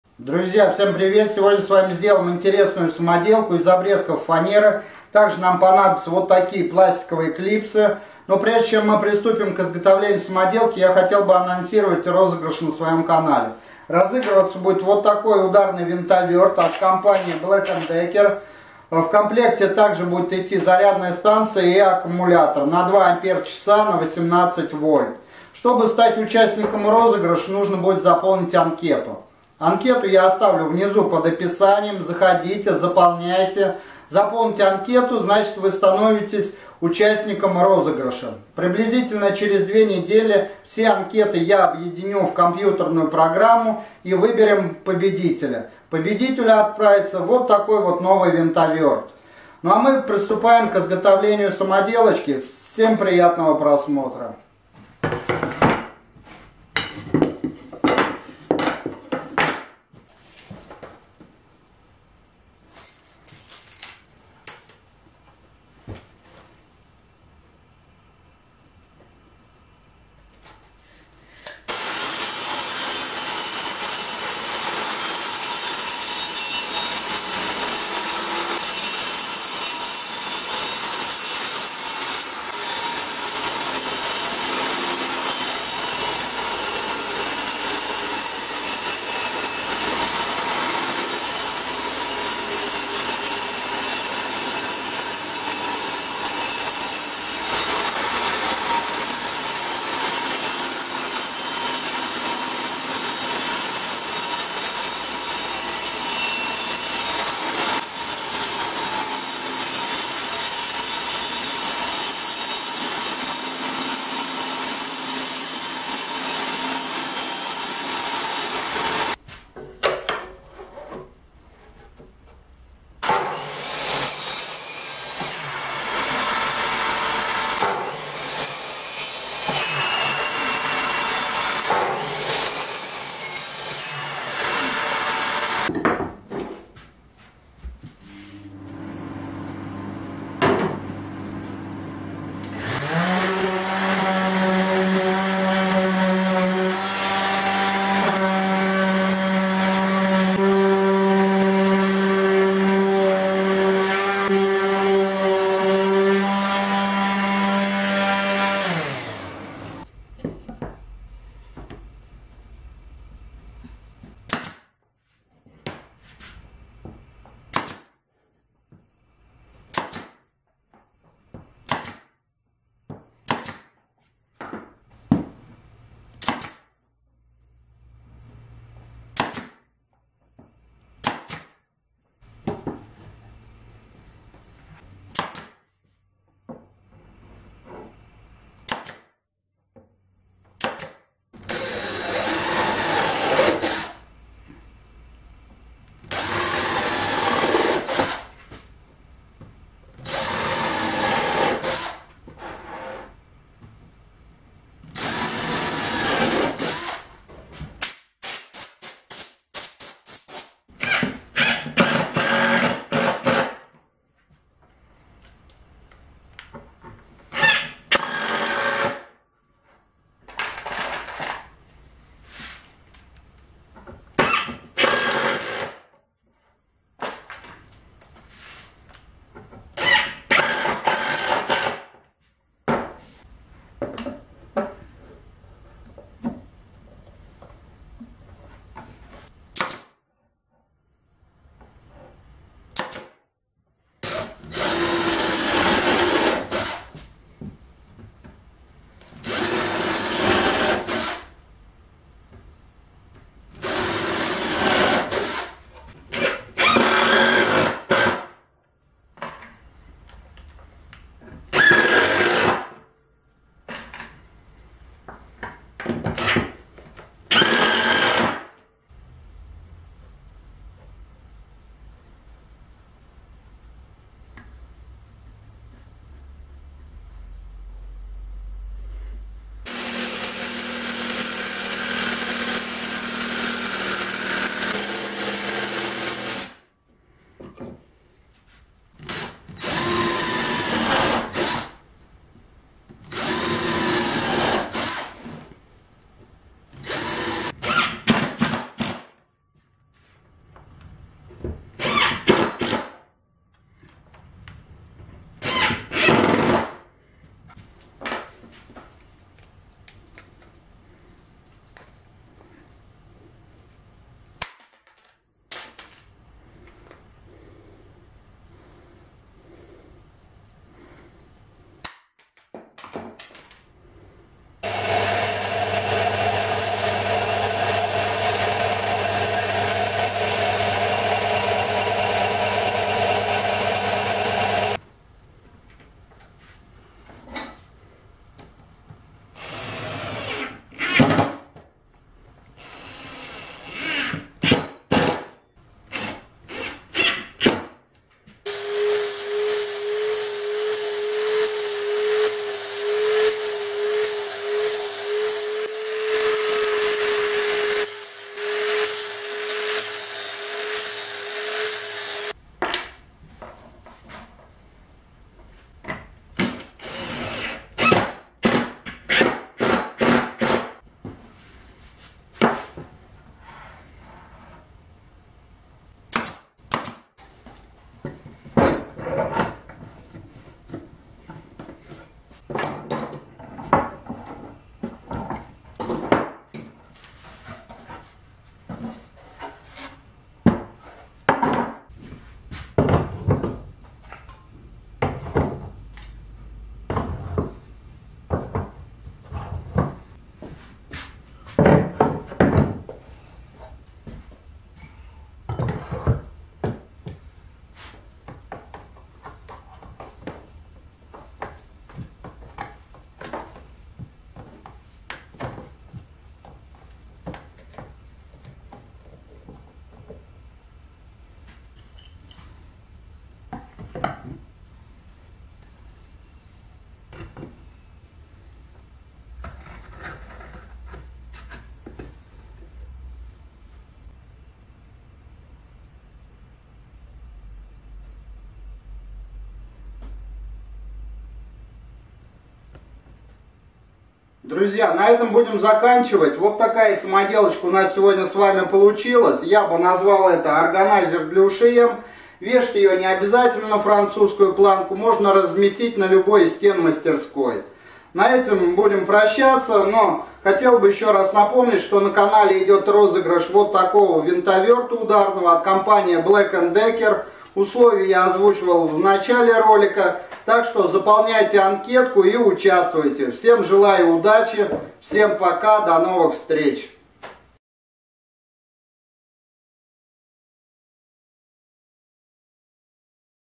УЗНАВ ЭТОТ СЕКРЕТ,ты больше никогда не выбросишь СТАРУЮ КЛИПСУ! Не трать деньги,сделай сам!
Секрет, который хранится в старой клипсе! Практически ни кто его не знает!! Узнав этот секрет ты больше никогда не выбросишь старую клипсу!